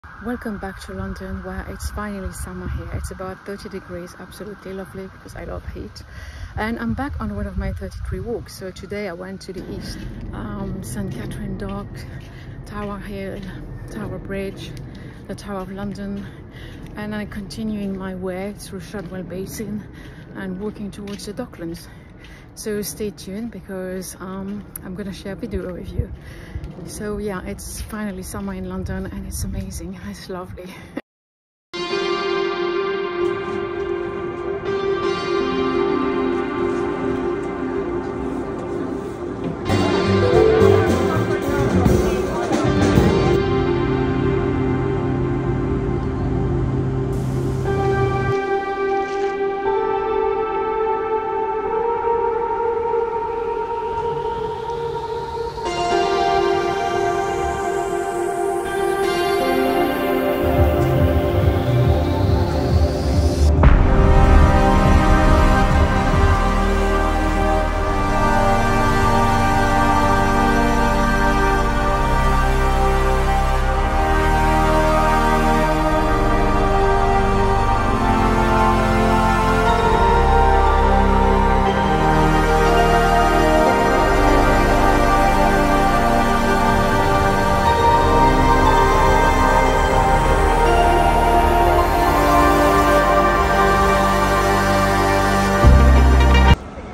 Travelling From St Katherine's Dock - Take a Walk Through the Docklands
Welcome back to my 33walksinlondonyoushoudnmiss. I went on walk number 17, the Wharfes walk to the East of London. I’m unfamiliar with the area, but I am still surprised by pubs from the 16th century standing in the middle of modern buildings or arriving in St Katherine’s Dock during a celebration for the small boats that went over to Dunkirk. Without the guide, I would never have uncovered so many of the @111places. This video invites you to rediscover your surroundings and places with which we are all too familiar. Go again and again and will uncover incredible stories.